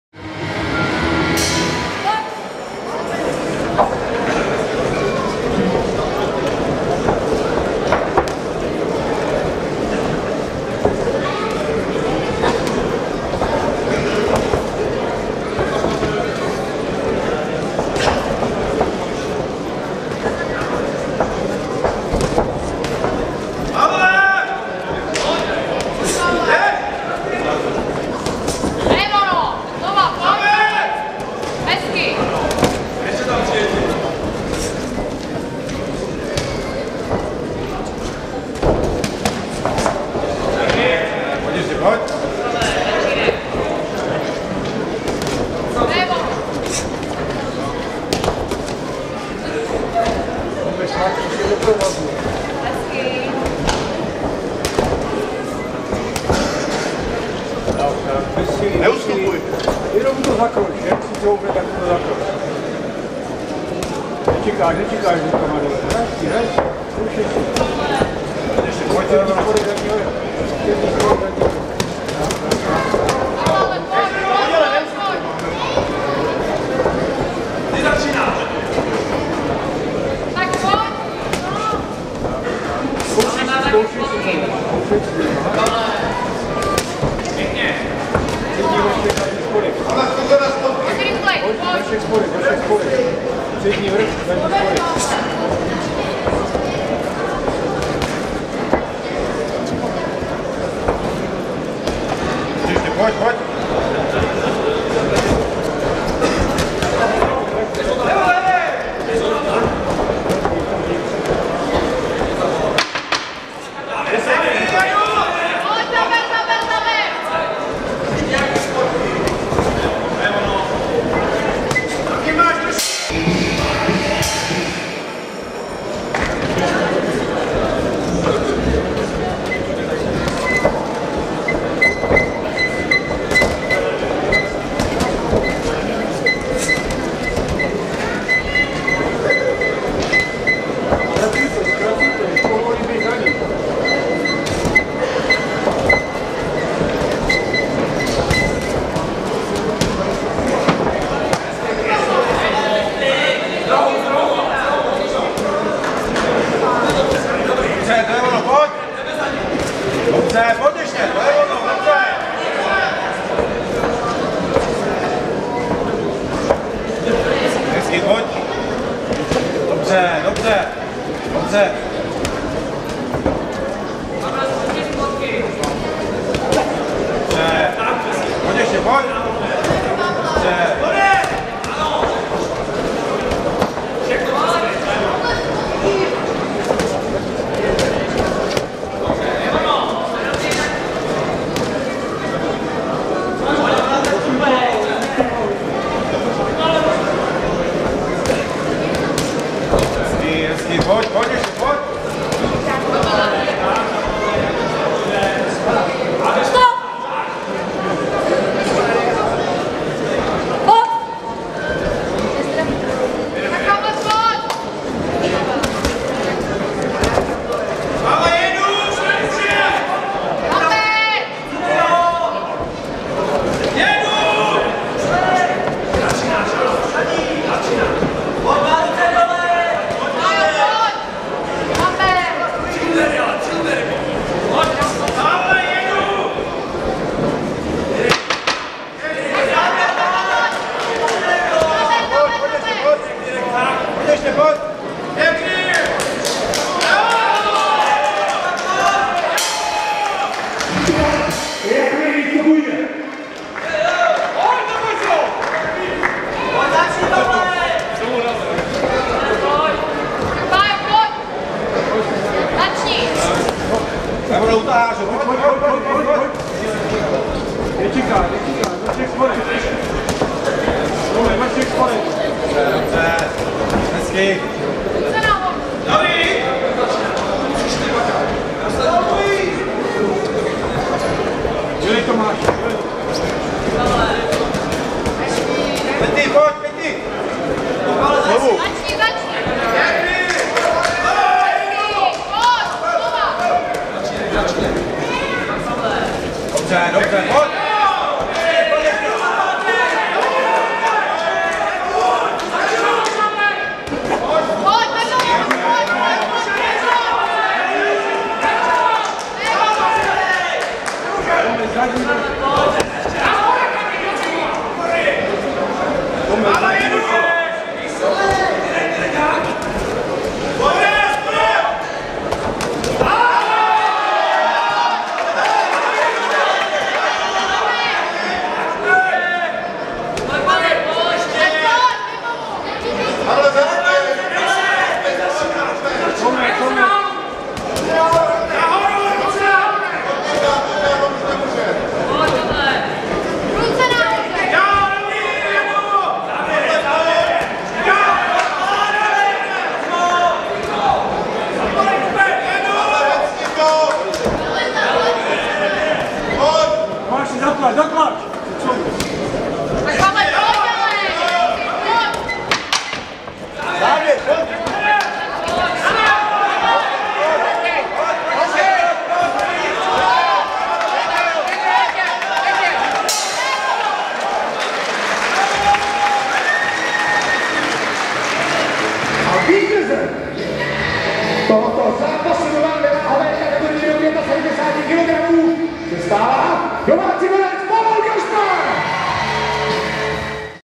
Pavel Kašpar vs Tomáš Hradečný, Boxing Poděbrady
Zápas z Galavečeru boxu v Poděbradech, konaný 8.2.2015 v poděbradské hale Bios. Pořadatel Boxing Poděbrady, Daniel Vencl